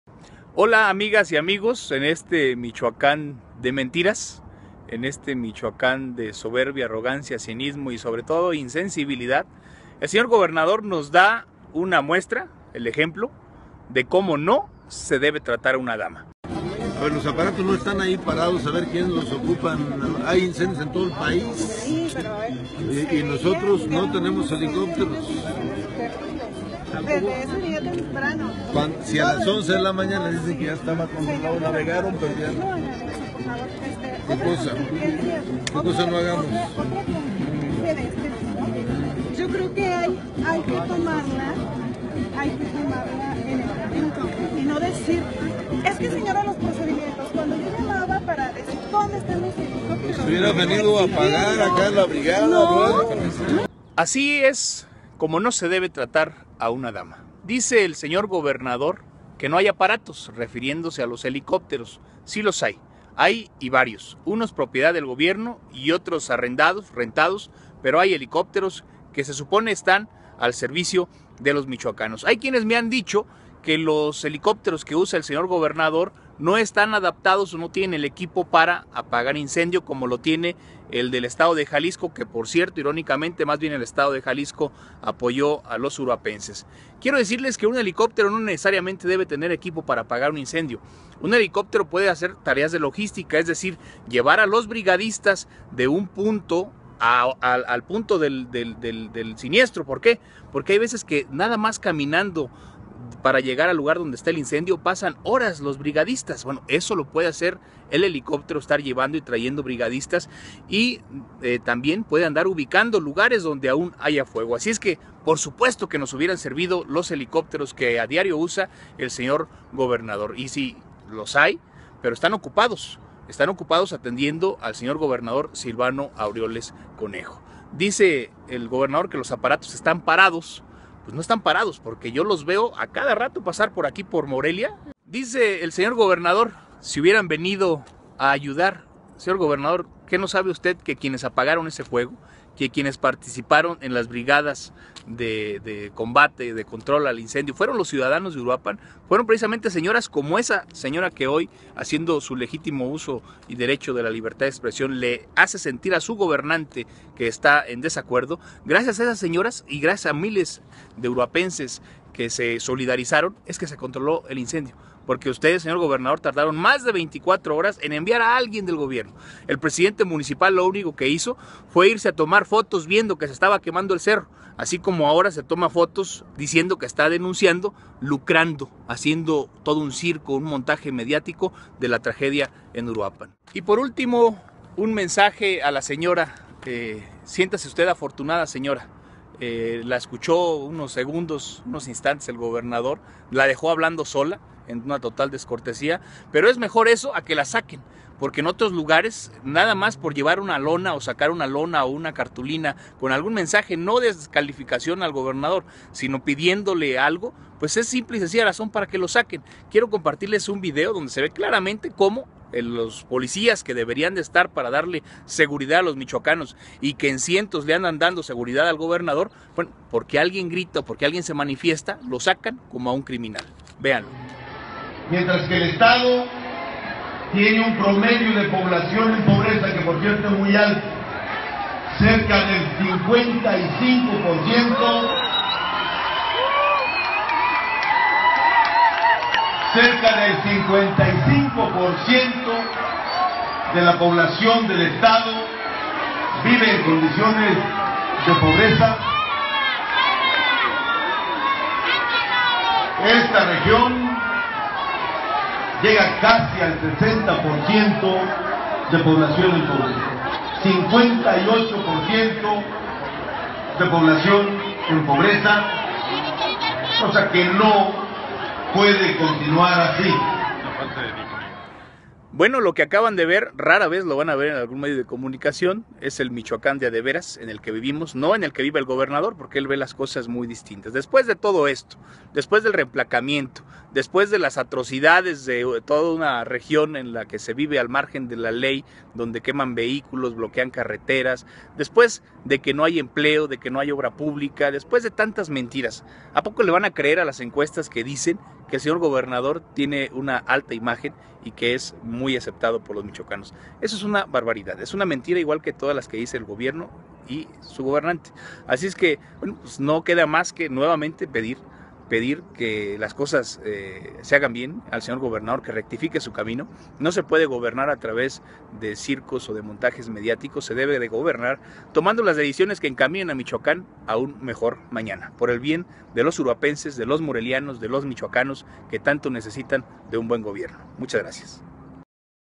Vídeo ecocídio en Uruapan e insensibilidad de Silvano Aureoles
Análisis de las mentiras del gobernador Silvano Aureoles en torno a la ausencia de apoyo en el ecocidio de Uruapan.